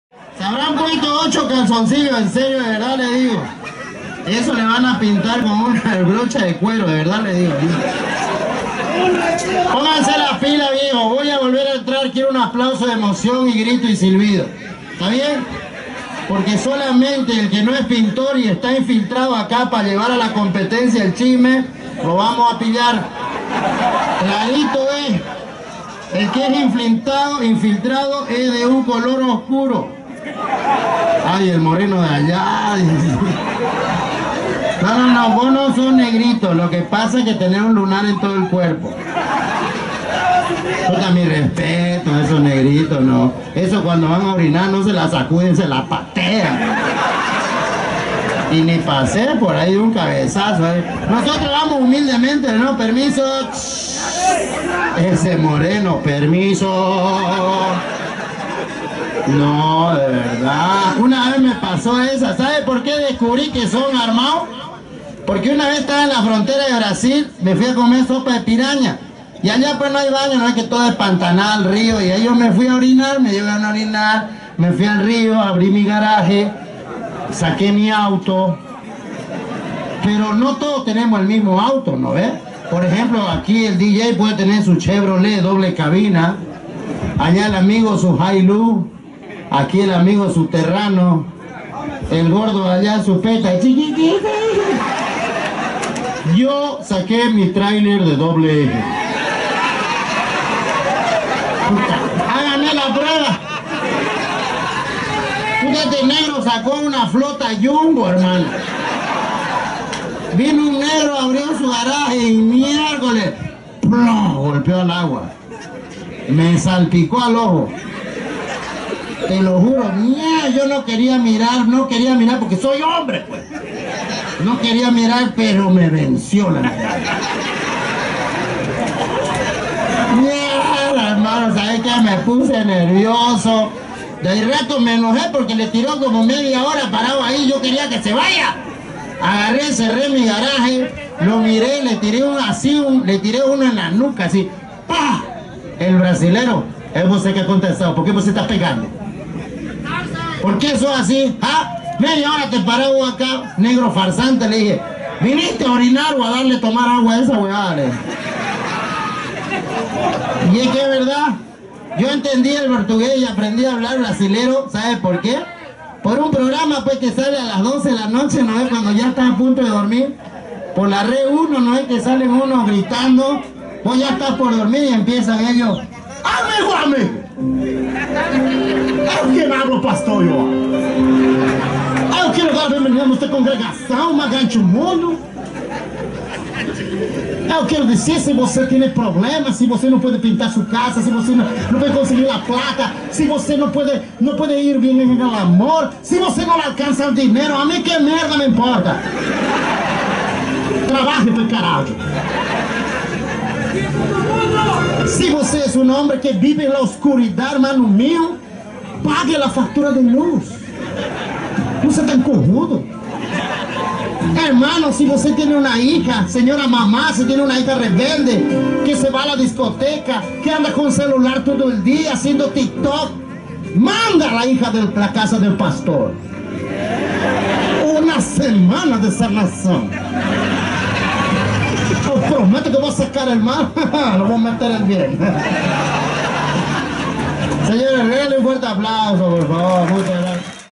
GORDO TV - PRESENTACIÓN RIBEPAR 2022
presentación oficial del gordo tv en el día del Pintor 2022, del grupo Ribepar